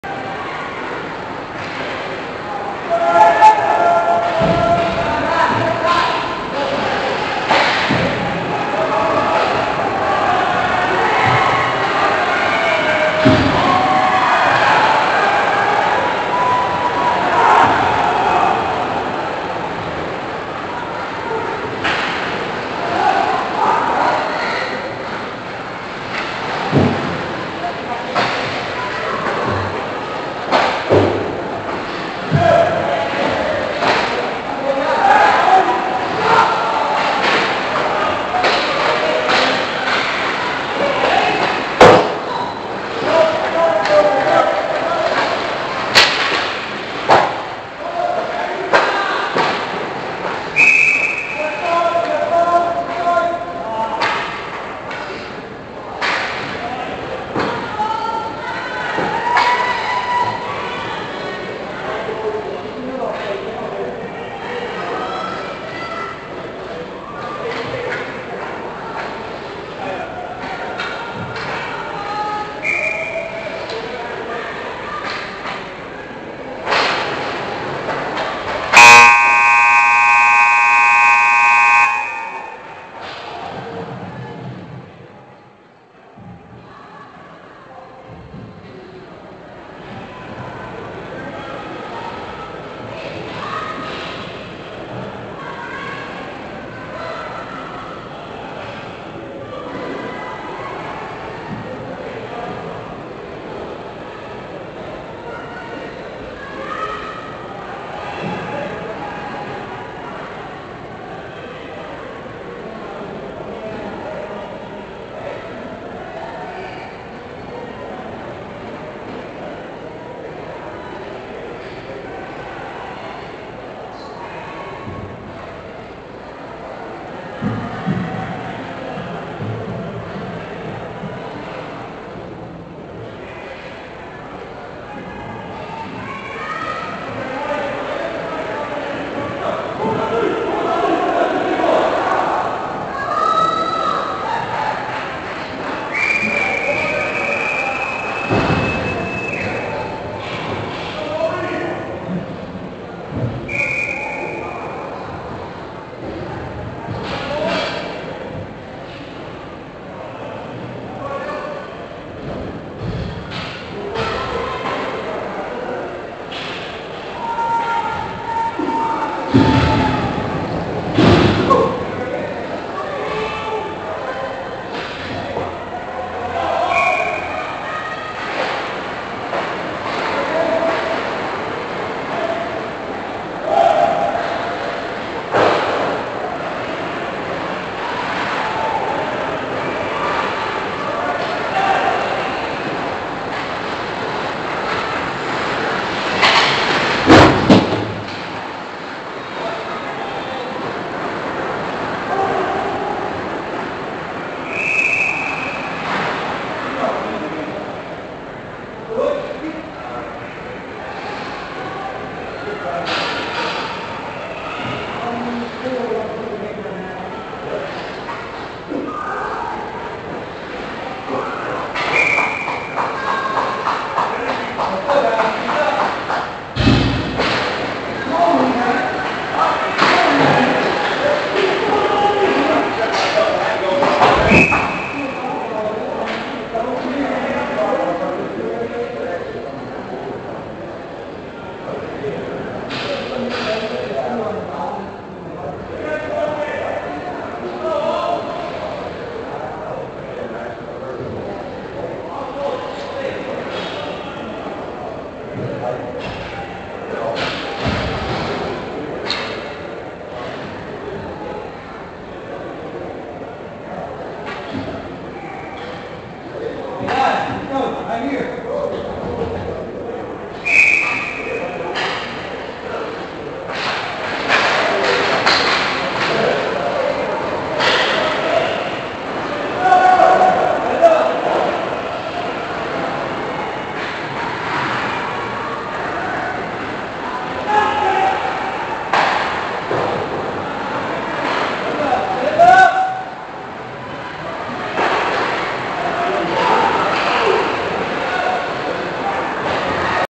BLACK - Tampa Palms Hockey - IceHawks vs Brandon Bulls RR - 9 of 14
Tampa Palms - Icehawks vs Brandon Jr. Bulls Round Robin Game 2 Midget A - Big Bear Tournaments - Ice Hockey